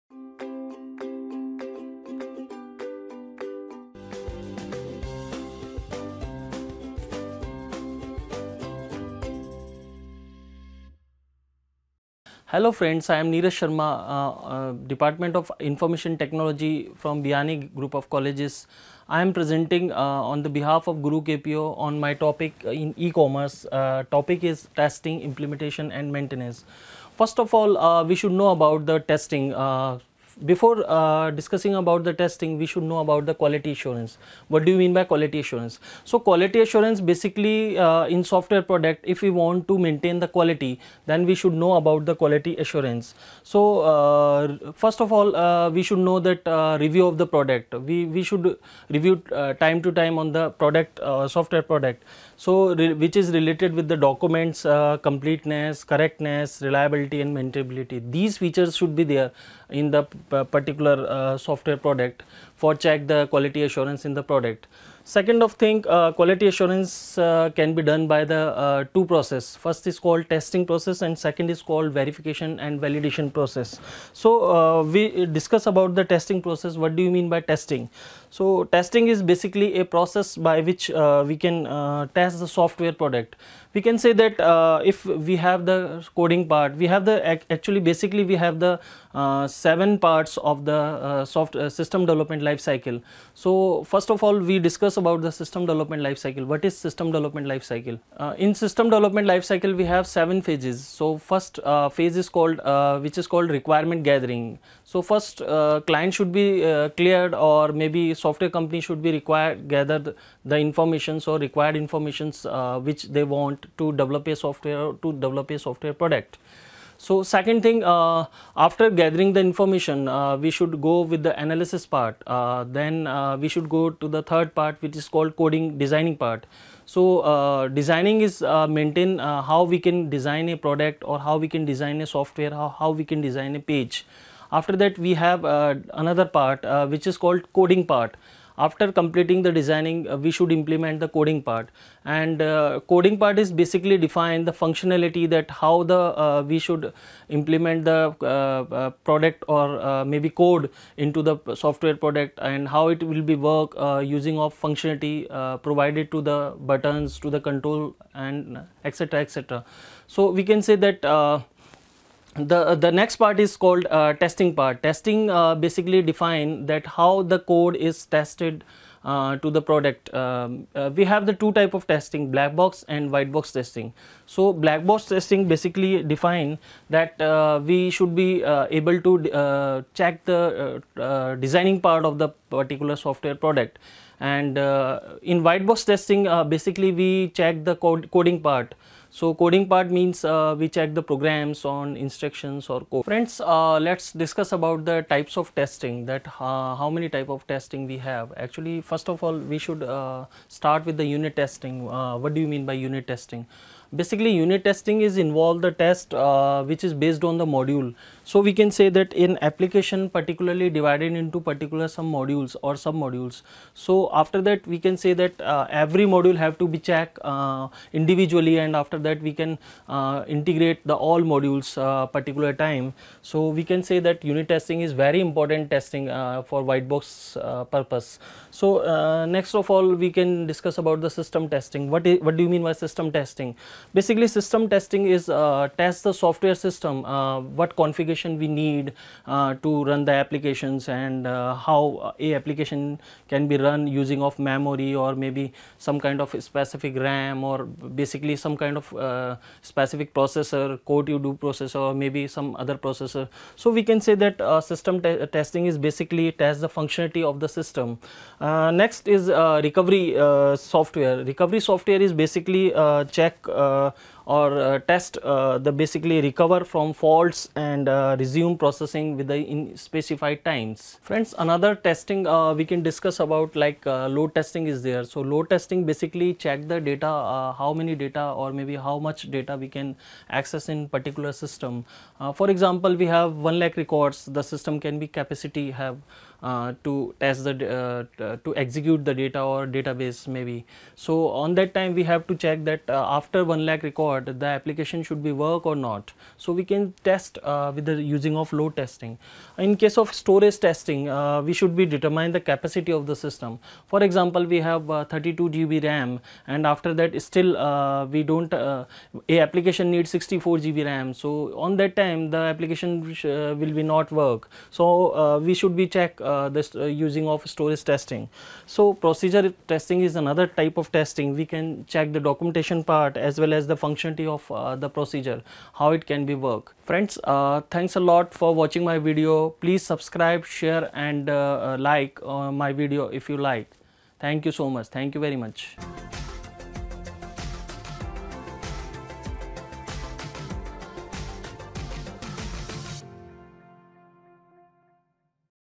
Testing Implementation and Maintenance(BCA, MCA)
Assistant Professor Neeraj Sharma explained about Testing Implementation and Maintenance

Gurukpo.com is the fastest growing educational web portal where all kind of academic information/Notes are available free of cost.